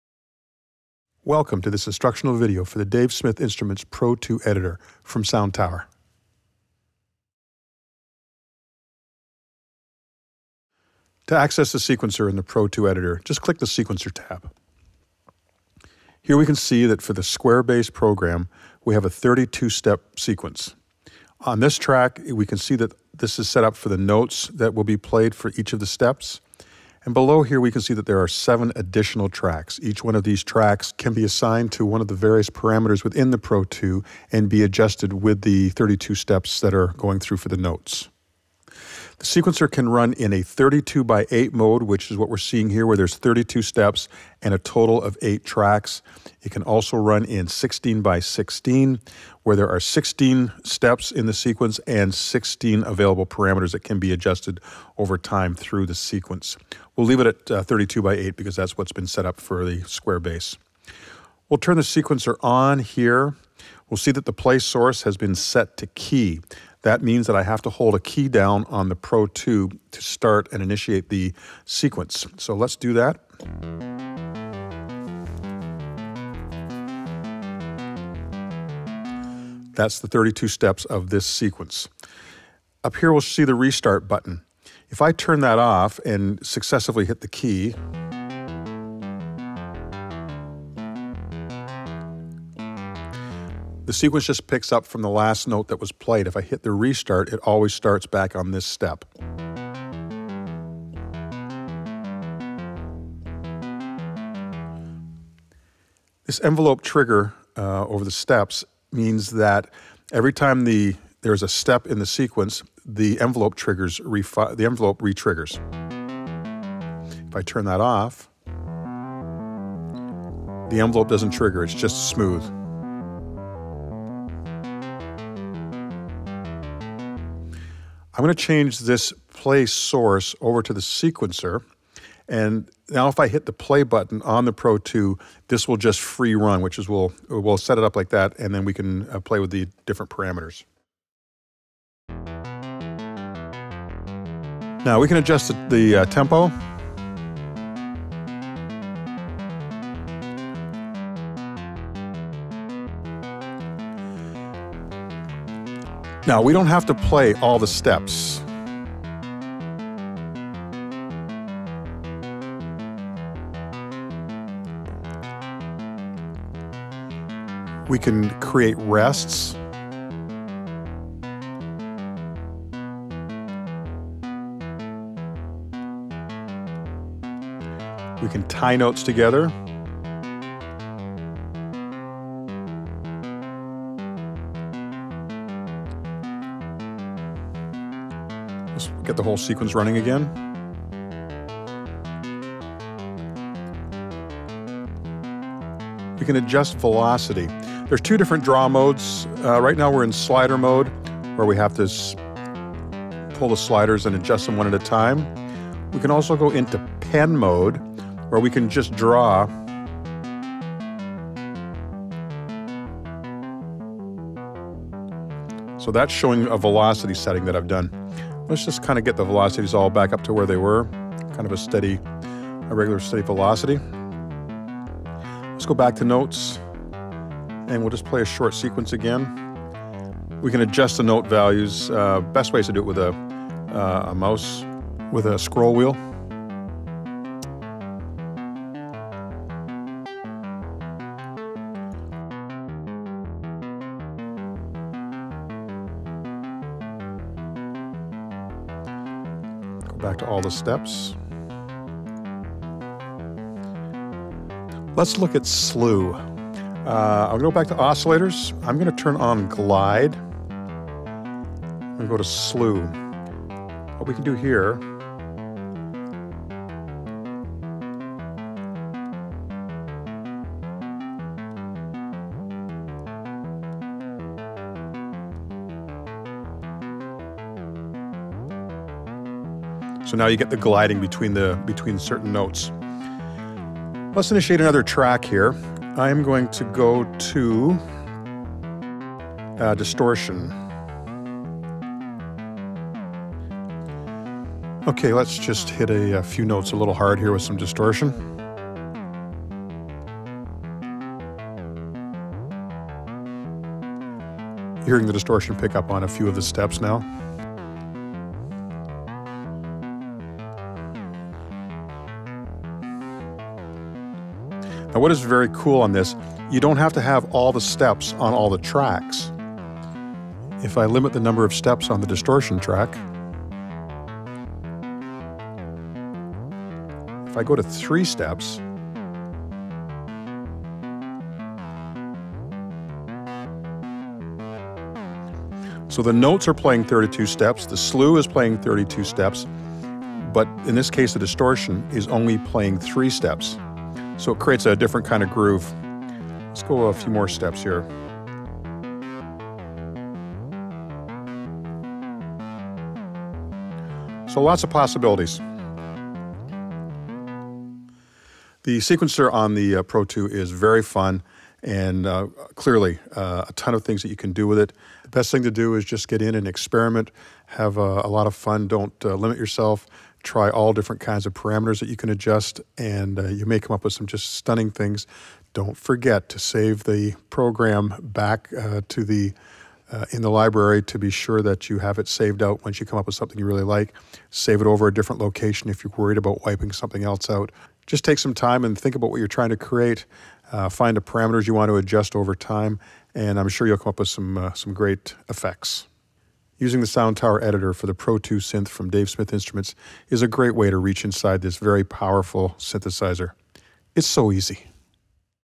Soundtower's Sound Editor for DSI PRO 2 - Sequencer
Helpful guide to use and edit 32-step 8-track Sequencer in Soundtower's Sound Editor for DSI PRO 2.
Includes:
- Sequencer Settings
- Note, Velocity, Slew, Tie
- Editing individual Sequencer tracks  in many different ways.